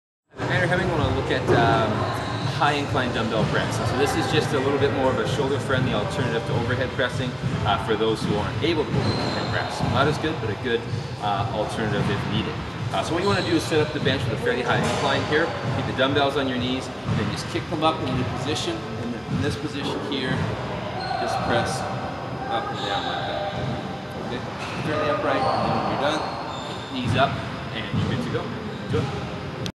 High Incline DB Press
I show an overhead press variation of pressing from a high incline position. This may be more shoulder friendly for some people.